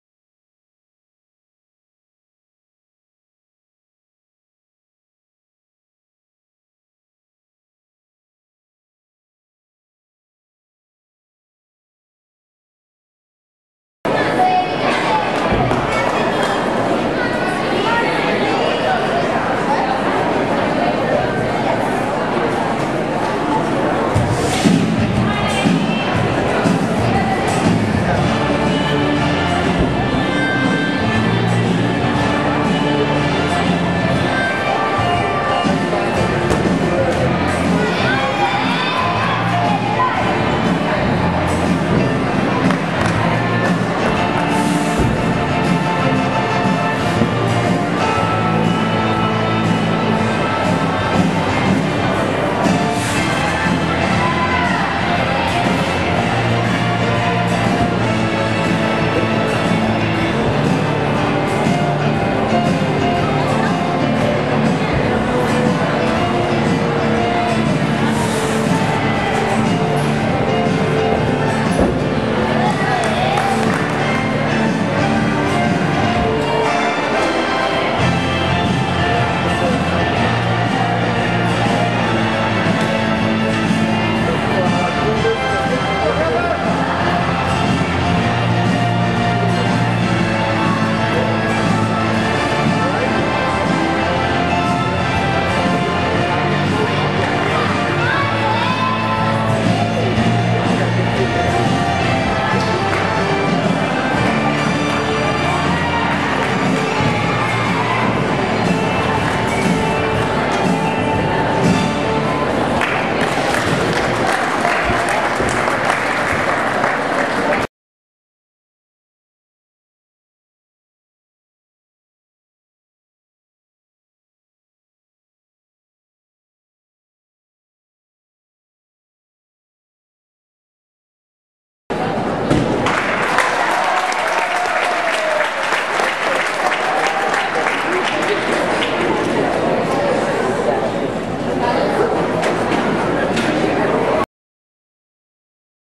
Sydnie Dillard Level 10 Texas State Championships Floor 2012
Sydnie Dillard Level 10 Texas State Championships Floor 2012. Score 9.375, Placed 7th